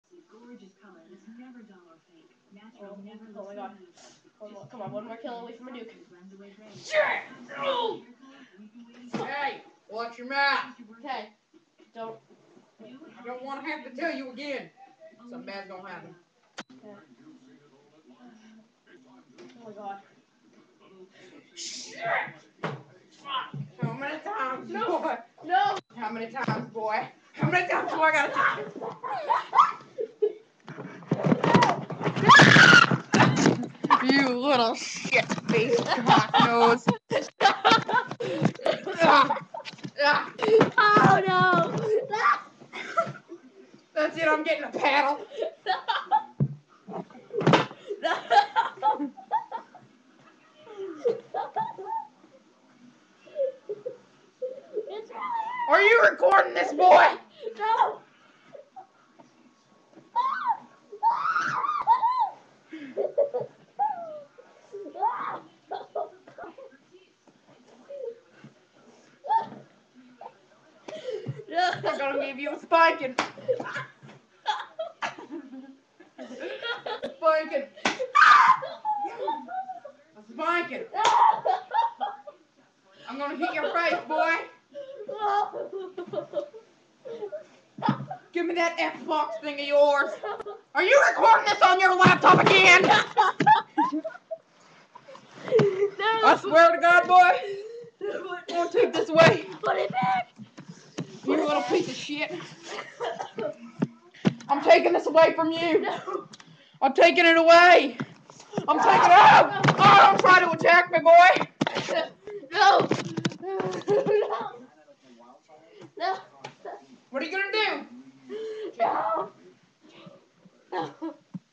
Getting beaten because of Modern Warfare 2
A boy gets abused in the first degree!!